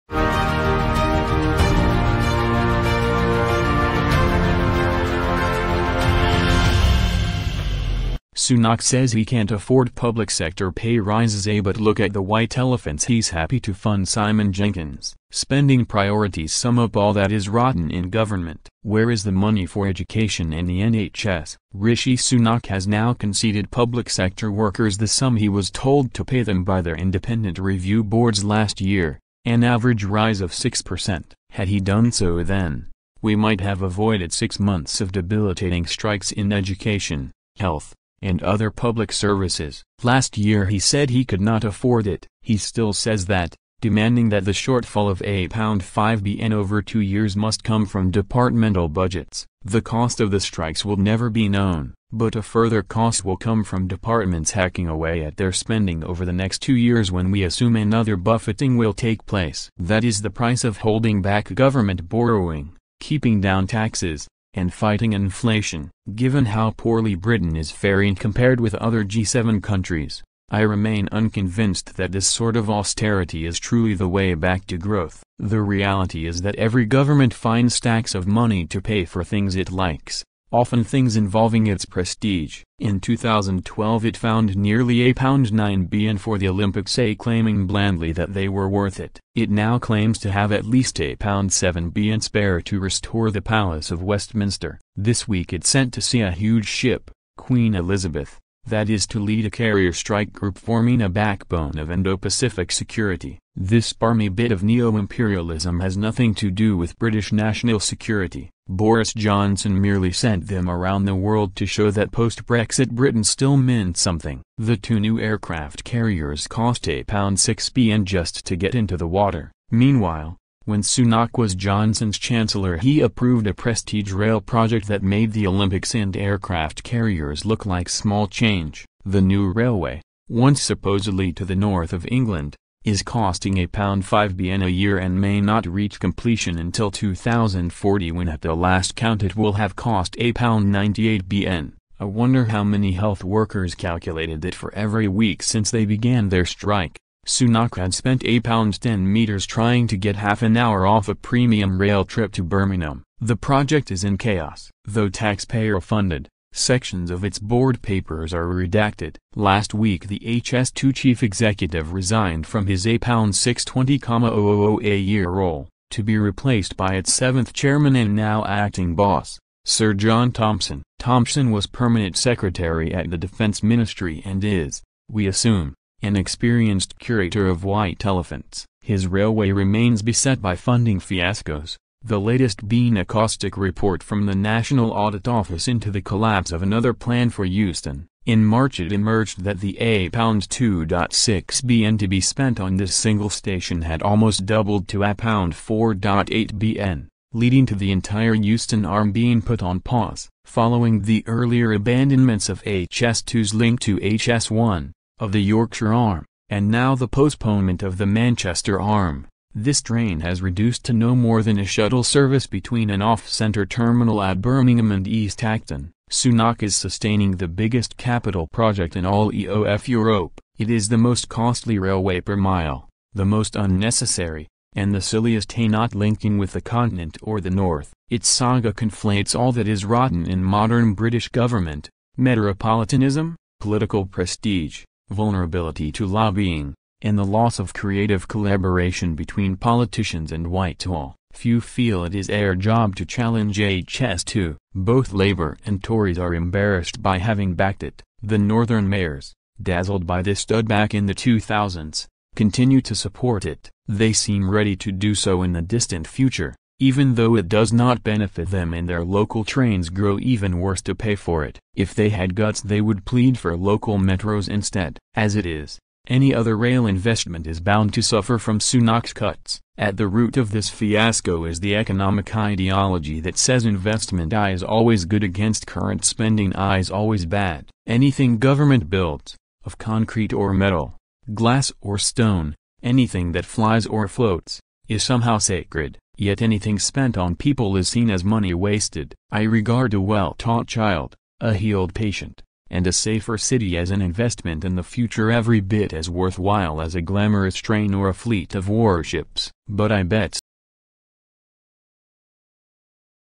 Sunak says he can’t afford public sector pay rises – but look at the white elephants
Welcome to The Multi News Tv.

This video is about informative news

In this channel, I provide you the latest and fresh news.